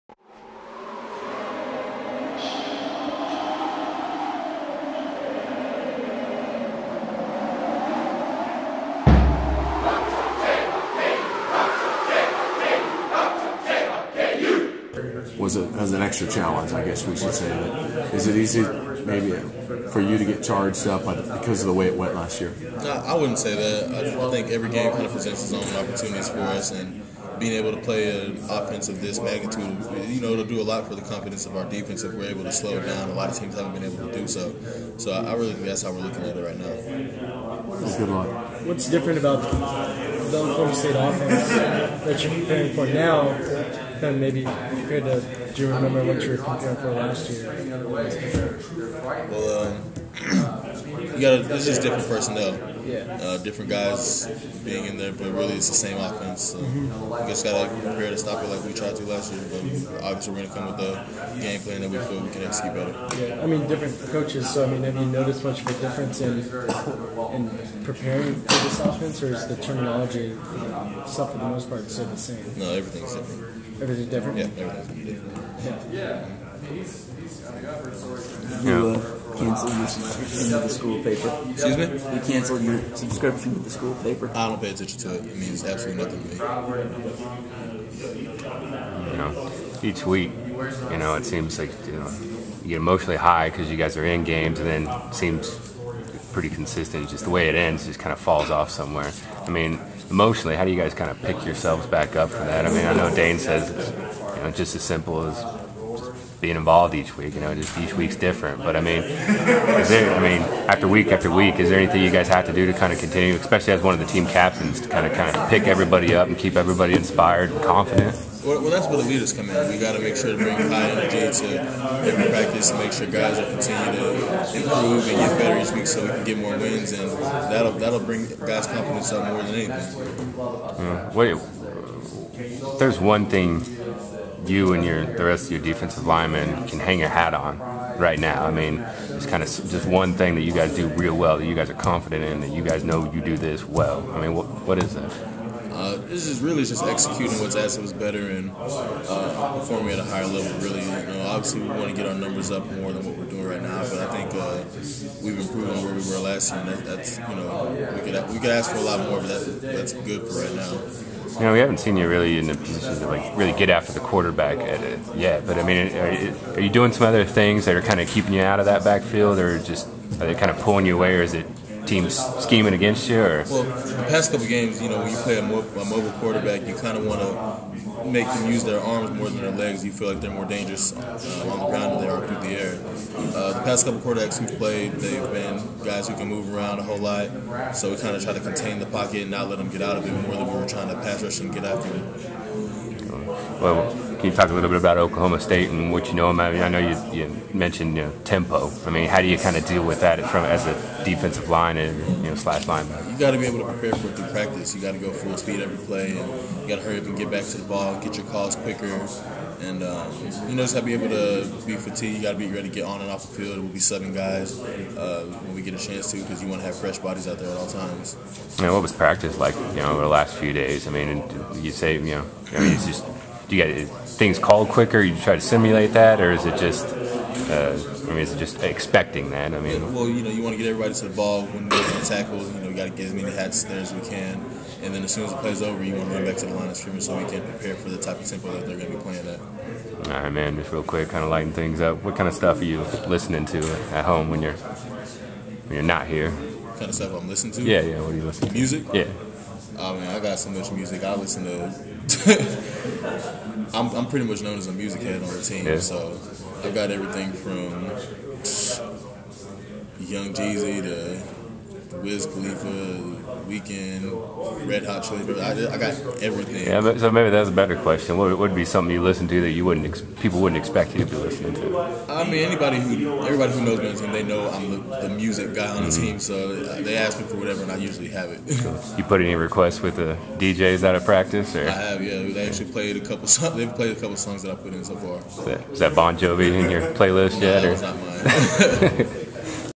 Toben Opurum 10 11 12
Kansas DE/LB Toben Opurum talks prior to facing Oklahoma State Saturday.